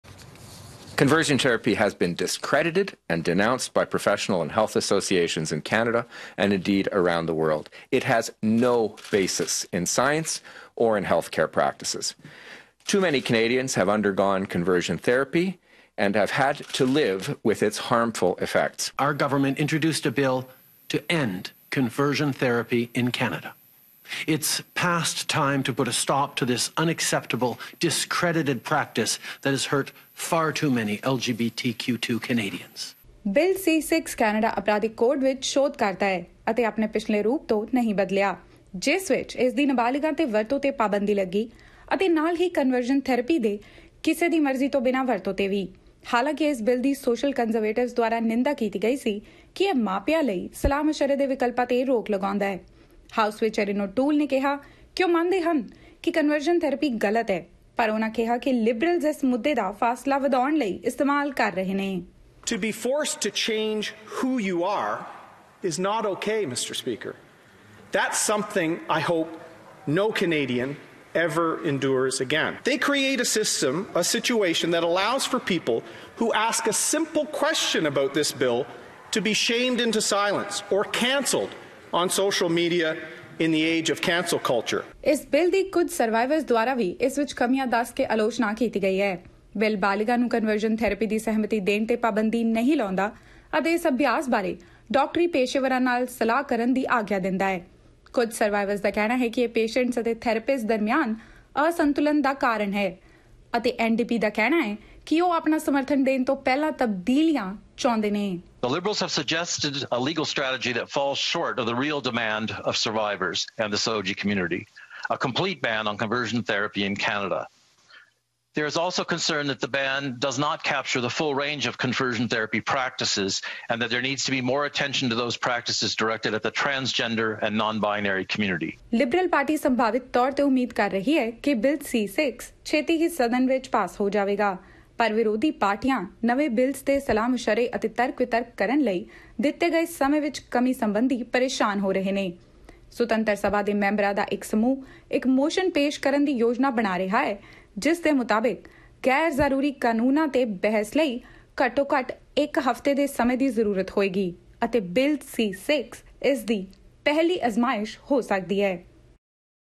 Ban on Conversion Therapy
Conversion therapy is still prevalent in Canada. But some experts say it can be harmful to mental health. The liberal government, which recently passed Bill C-4, will introduce another bill that will ban this therapy across Canada.